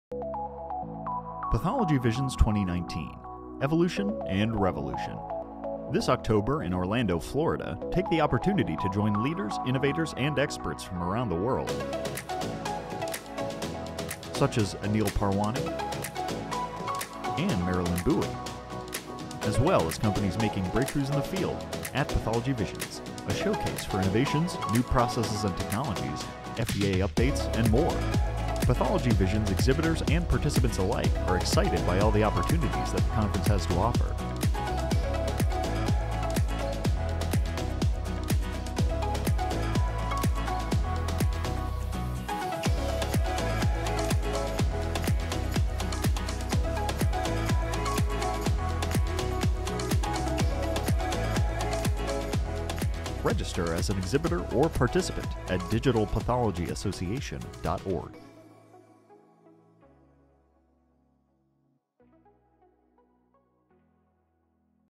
Pathology Visions 2019
The Pathology Visions Conference will give you the opportunity to learn about real-world, practical applications in the ever-evolving field of digital pathology. The Digital Pathology Association’s annual meeting will give you direct access to industry leaders, the opportunity to earn CME credits, your choice of cutting-edge workshops with distinguished trade visionaries, the chance to see the latest product solutions, and much more.